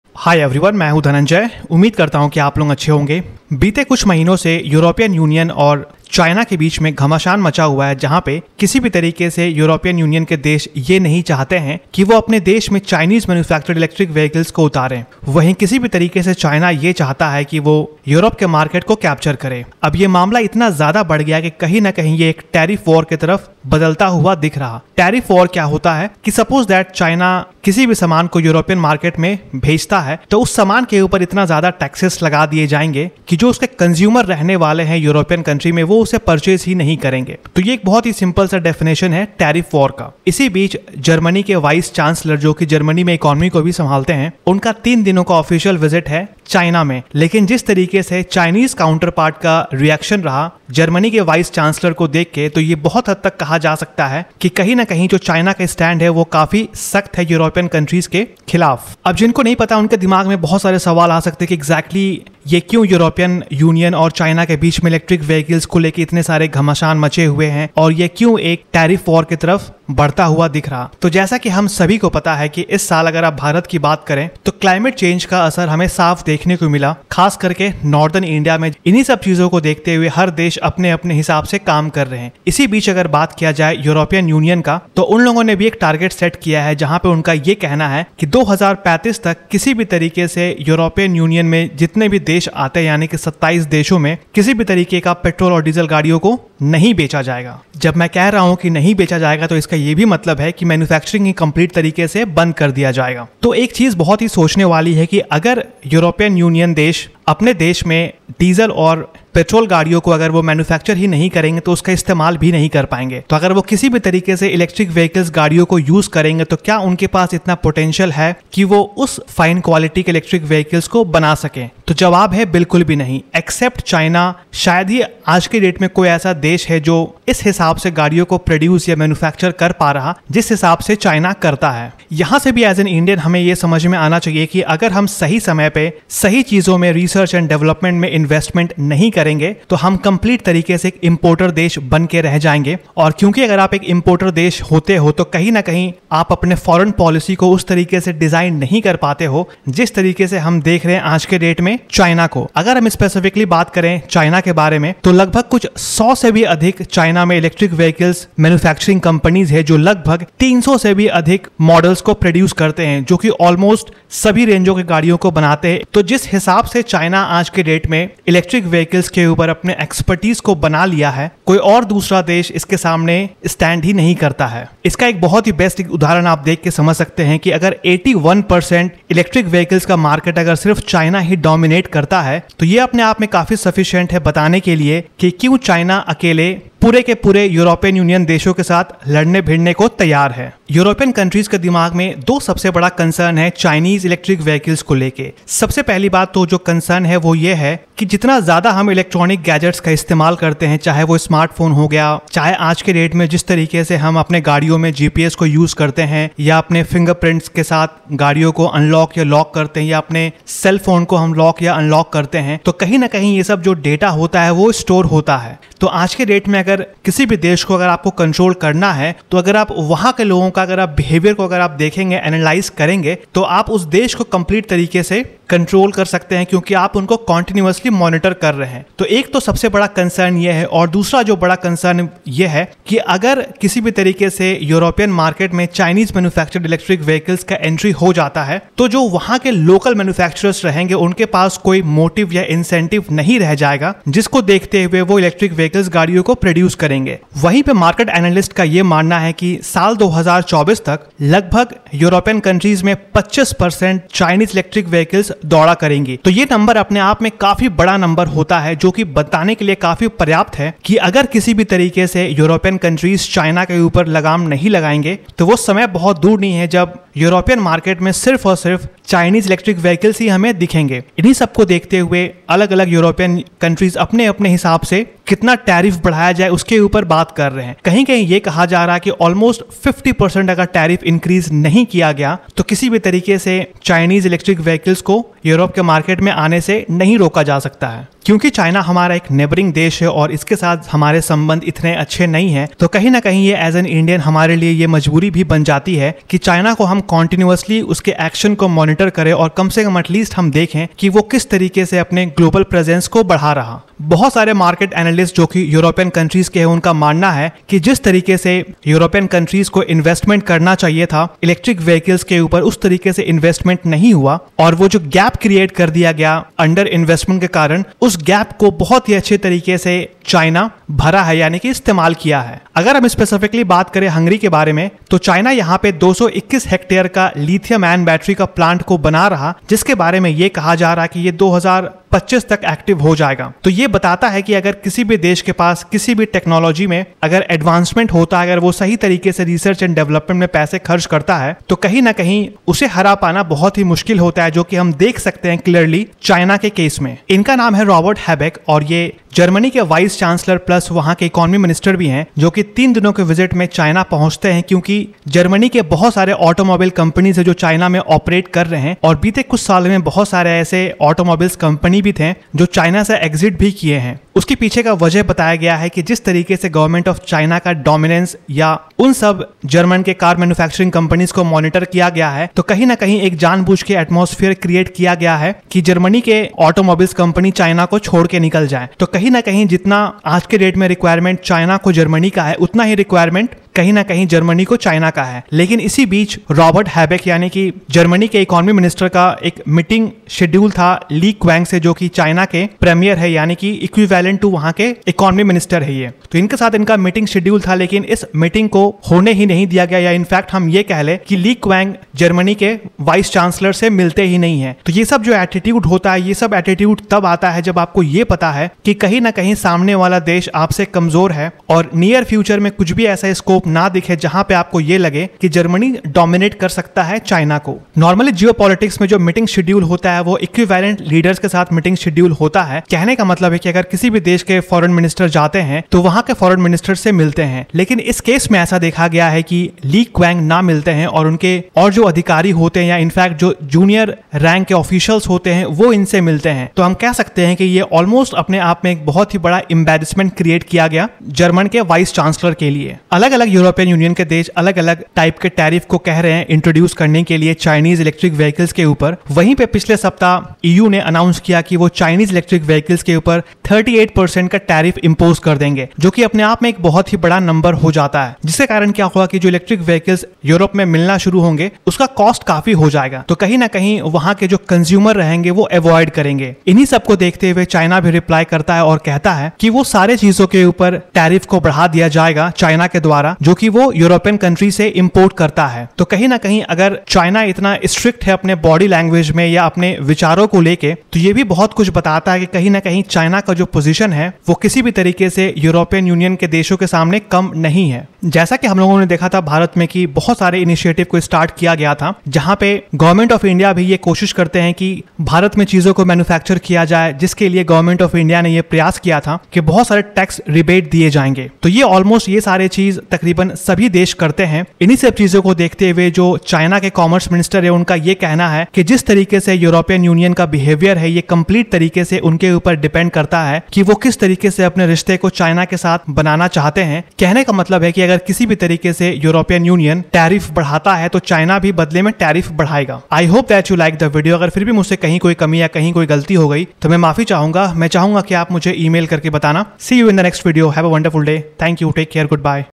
China Warns European Union Of Possible Trade War
Amidst economic conflicts and fears of a trade war, German Vice Chancellor Robert Habeck—who also serves as the nation's minister for economic affairs and climate action—reportedly failed to arrange a meeting with Chinese Premier Li Qiang during his three-day visit to China.

Before continuing on to Shanghai on Saturday, Habeck had planned to meet Li Qiang when he arrived in Beijing on Friday. However, Die Welt claimed that Habeck was told the appointment was "not possible in the morning before departure," and that he has "no idea" why the meeting was called off.